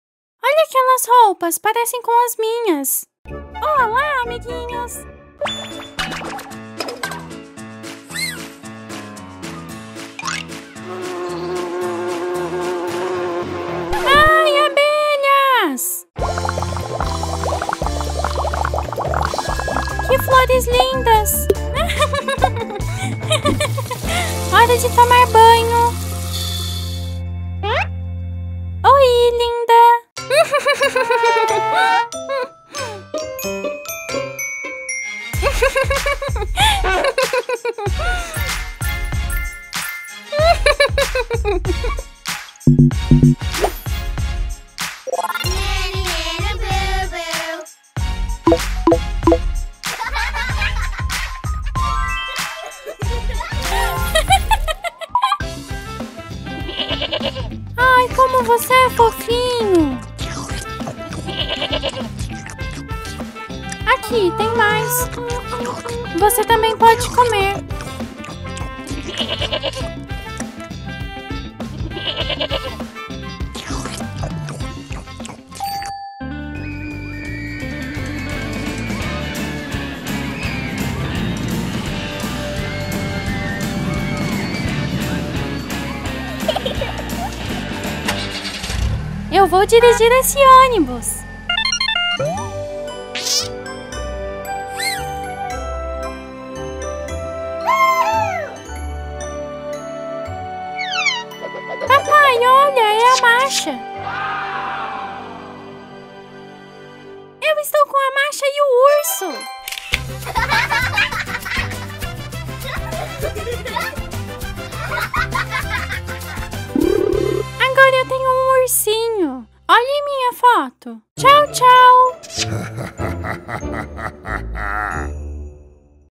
Stacy no parque de diversões Masha e o Urso
Stacy adora desenhos animados e ela veio ao parque temático de Masha e do Urso.